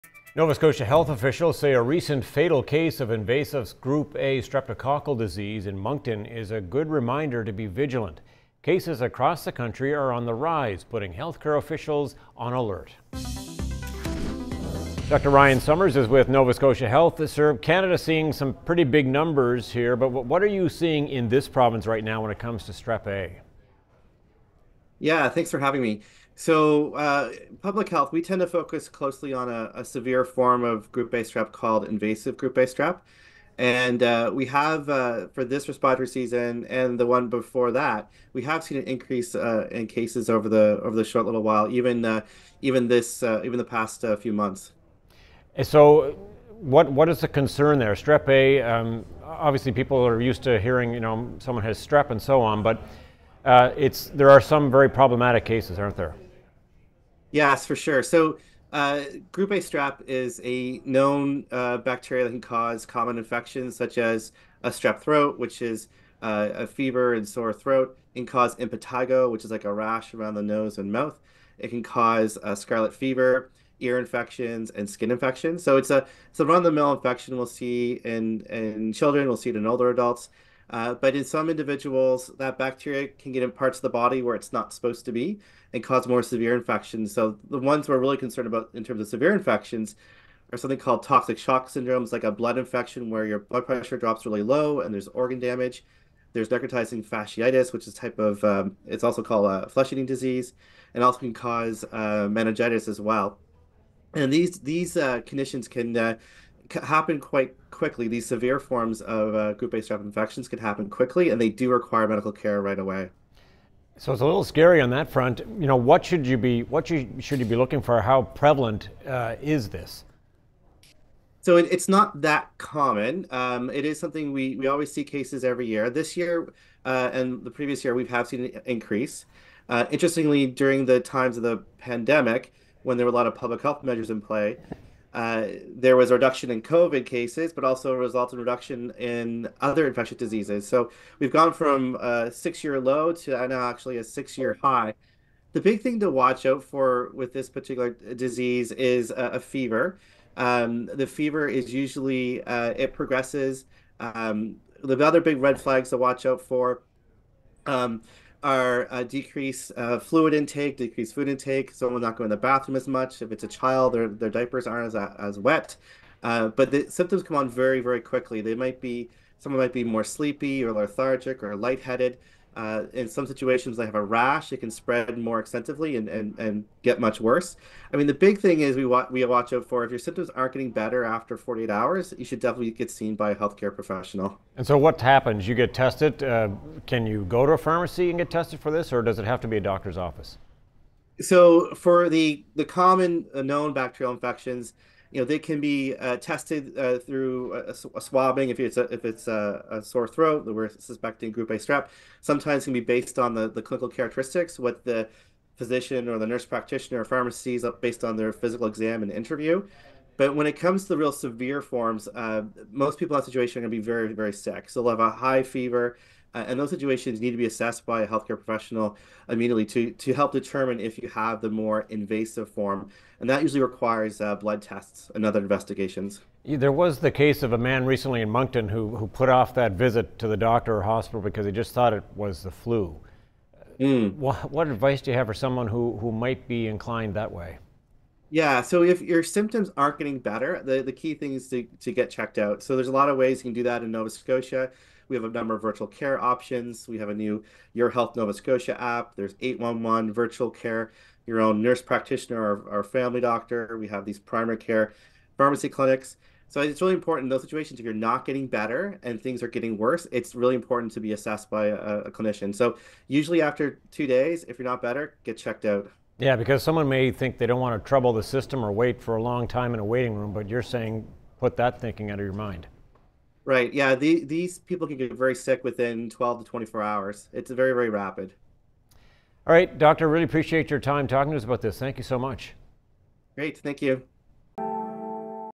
When to seek medical attention for strep throat
Strep throat is on the rise. Dr. Ryan Sommers with Nova Scotia Health says he is seeing an increase in the number of cases in Nova Scotia in the last few months. He says if you aren't recovering from strep throat symptoms within 48 hours, you should seek medical attention